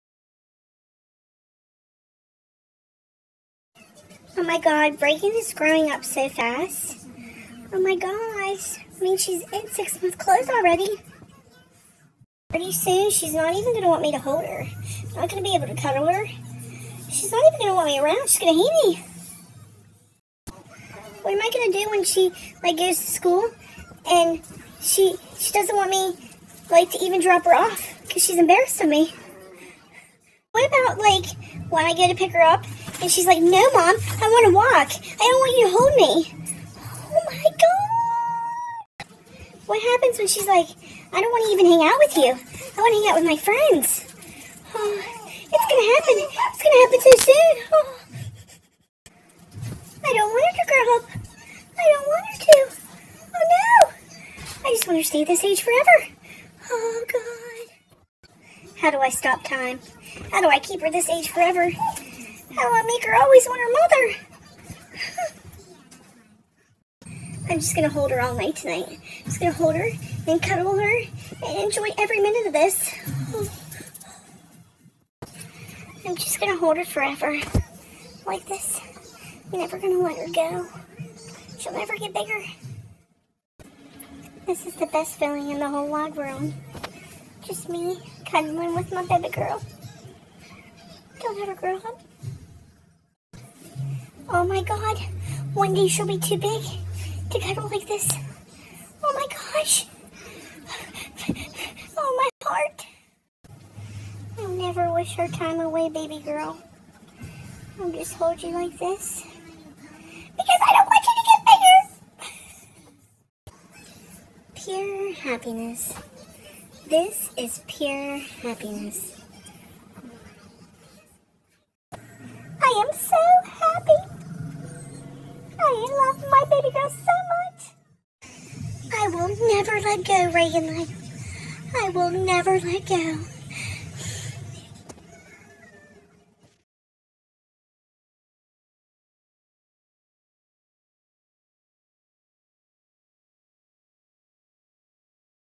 Mental Momma Ep. 3 Momma's Realization
Momma loses her stuff yet again ...